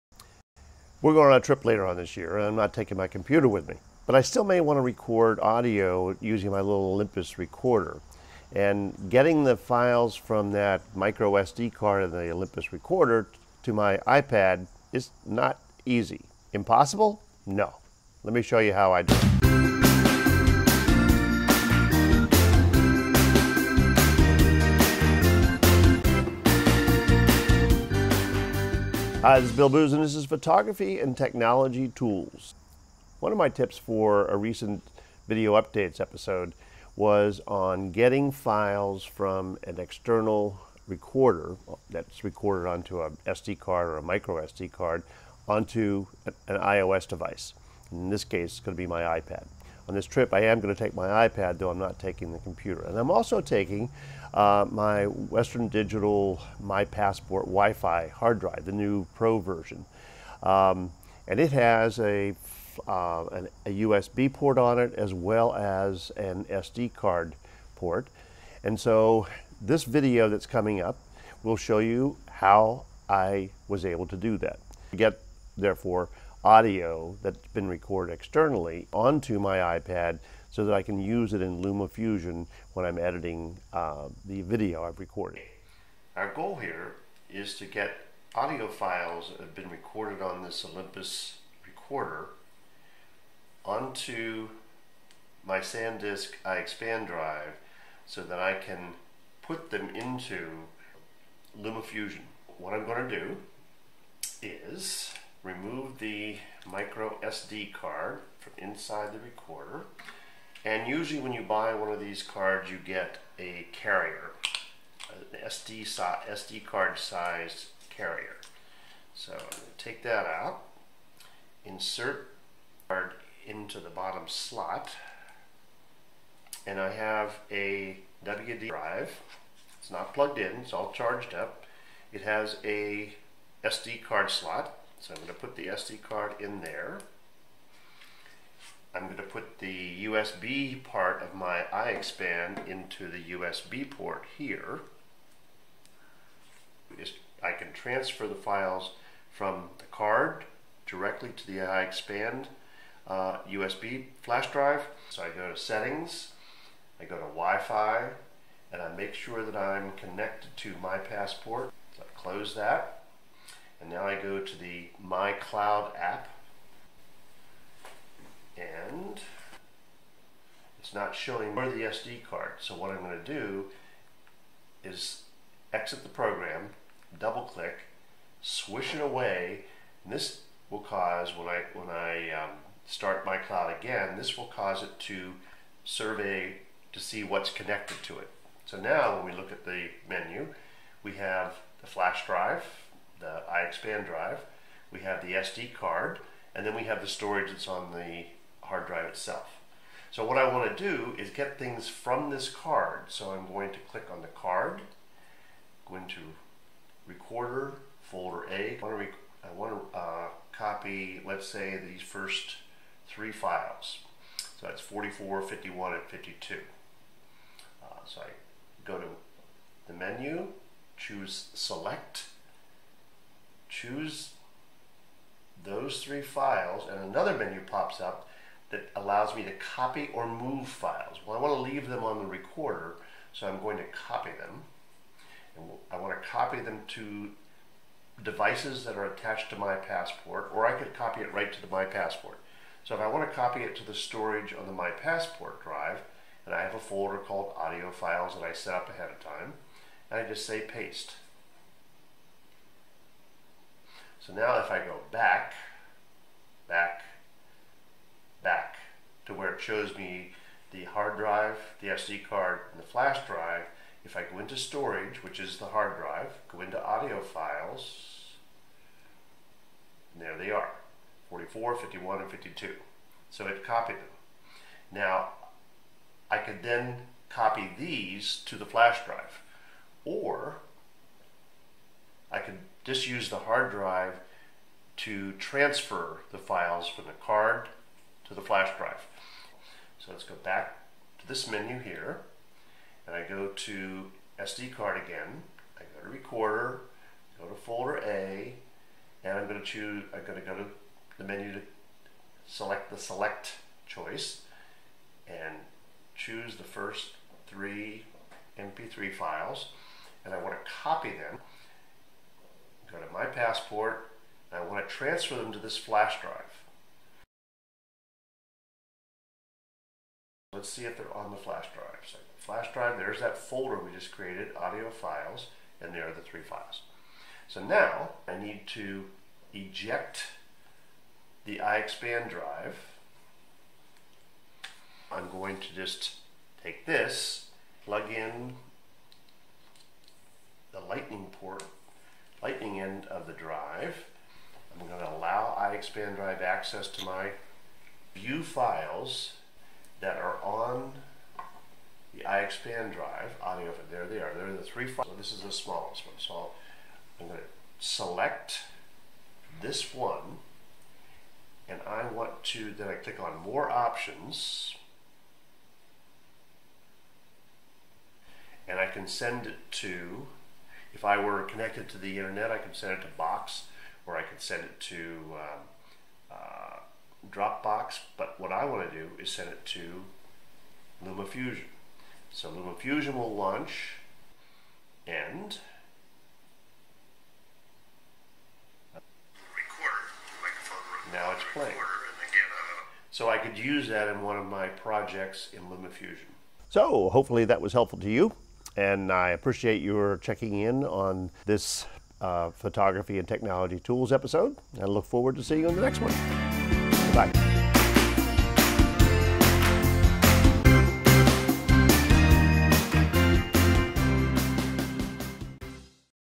PTT29 Getting Externally Recorded Audio Files Onto an iPad
Sometimes I need to record audio on my little Olympus audio recorder and then transfer the audio file to Dropbox or Box so I can import it into LumaFusion. But for that I use my computer connected to the Internet. If I don't have connectivity or don't have my computer with me - as I won't on an upcoming trip - then what can I do? In this video, I show you how I use my Western Digital MyPassport Wi-Fi Pro hard drive as the connecting point!